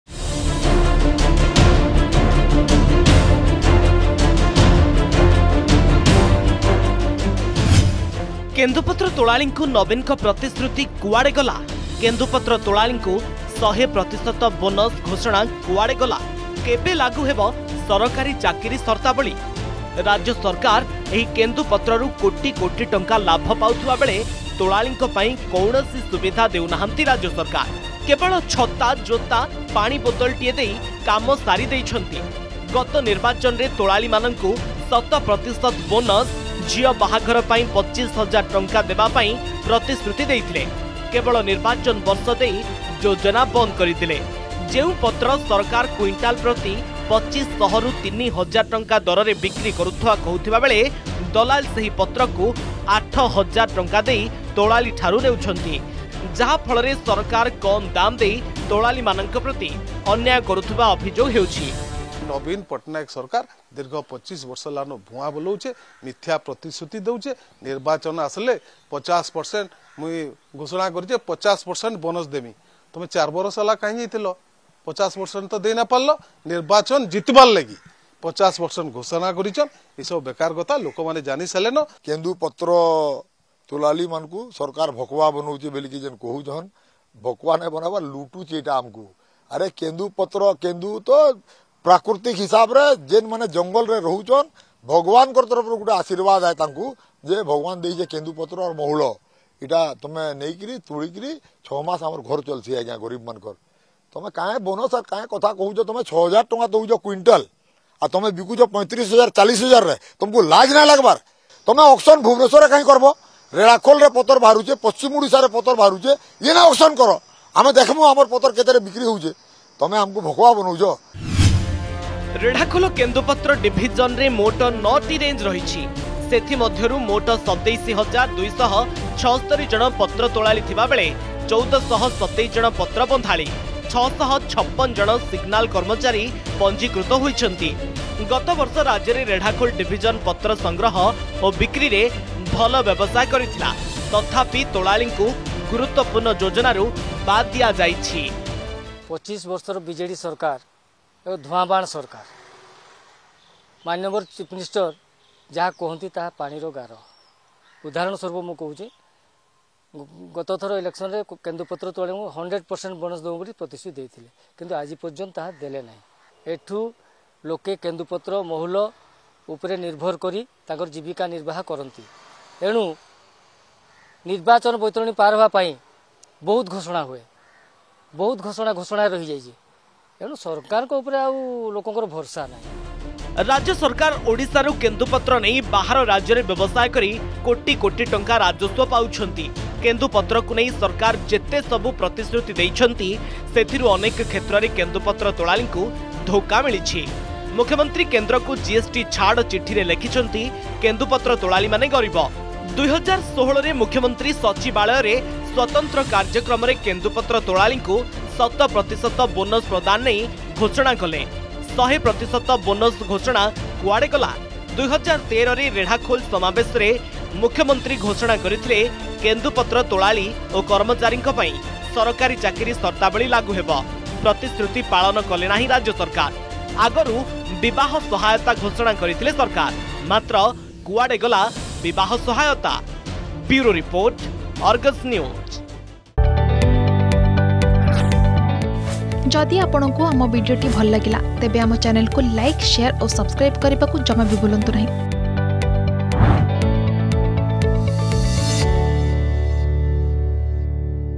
କେନ୍ଦୁପତ୍ର ତୋଳାଳୀଙ୍କୁ ରାଜ୍ୟ ସରକାରଙ୍କ ଅବହେଳା || Kenduleaf Scam odishagovt
କେନ୍ଦୁପତ୍ର ତୋଳାଳିଙ୍କୁ ନବୀନଙ୍କ ପ୍ରତିଶ୍ରୁତି କୁଆଡେ ଗଲା । କେନ୍ଦୁପତ୍ର ତୋଳାଳିଙ୍କୁ 100 ପ୍ରତିଶତ ବୋନସ୍ ଘୋଷଣା କୁଆଡେ ଗଲା ? କେବେ ଲାଗୁ ହେବ ସରକାରୀ ଚାକିରୀ ସର୍ତାବଳୀ ?